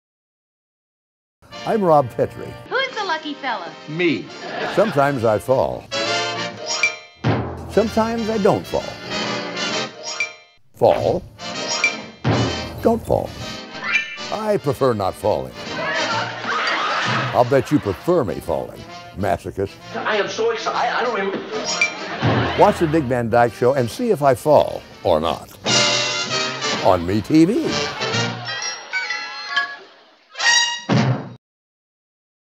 Dick Van Dyke "Ottoman Falling" Me-TV
Dick Van Dyke talks about the ottoman and whether to fall, or not to fall on "The Dick Van Dyke Show" for MeTV.

Shot this at Frank & Musso Grill in Hollywood the day of the Peter Falk Hollywood Walk of Fame Event on June 25, 2013.